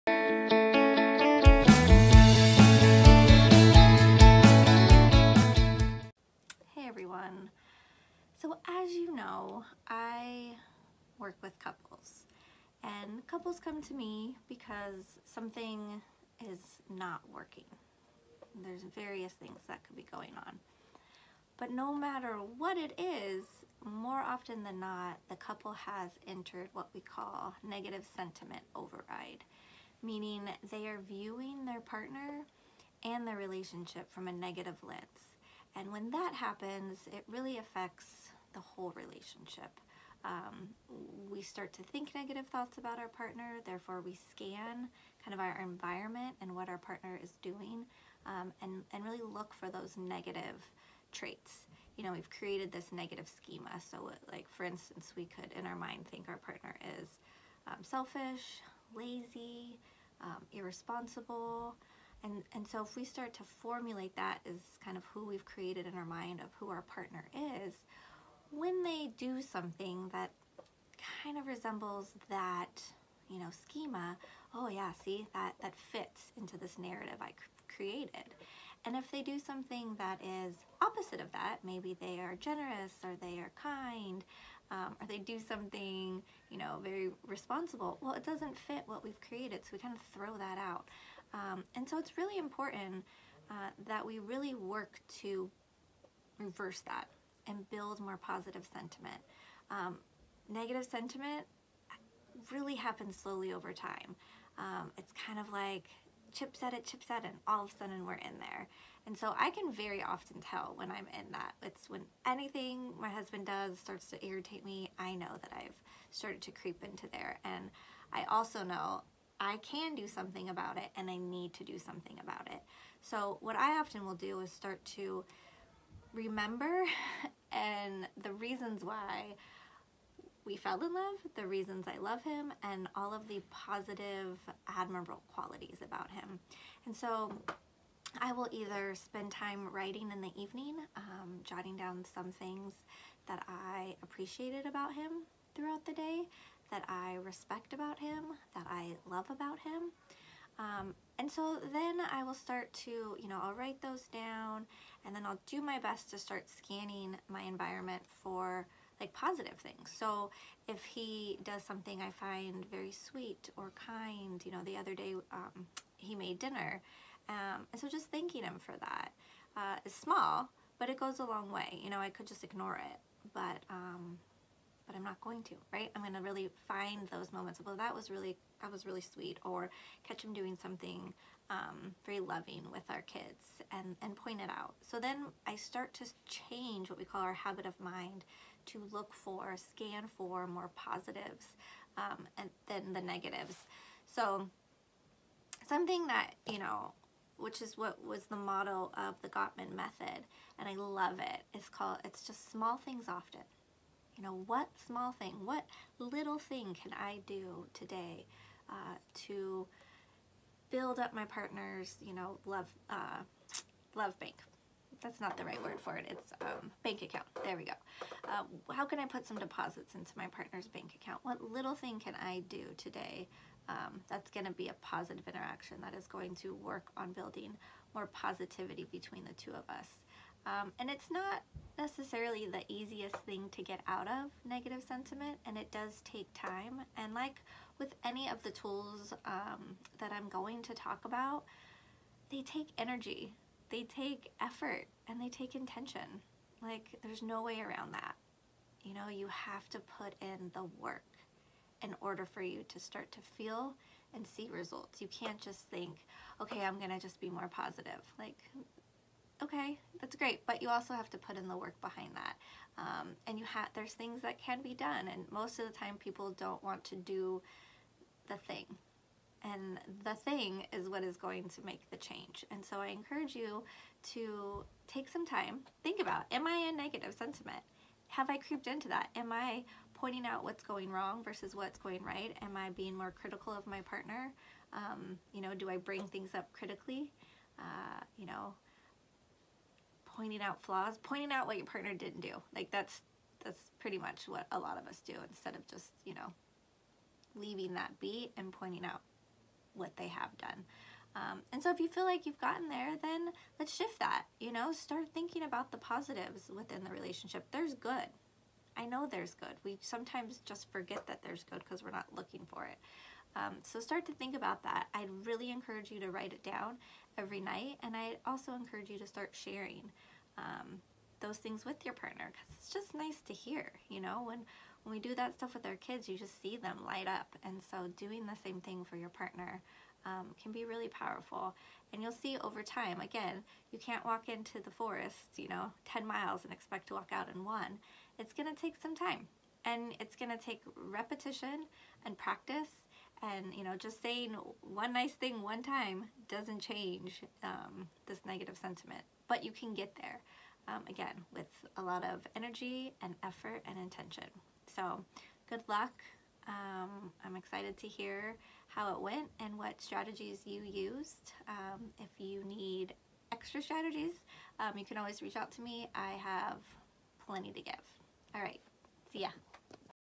Build Positive Sentiment
This video gives you tools to move your relationship out of negative sentiment override into a more positive space. Doing small things often will make the big changes you have been longing for.

To listen to my podcast Session with Beth Wylie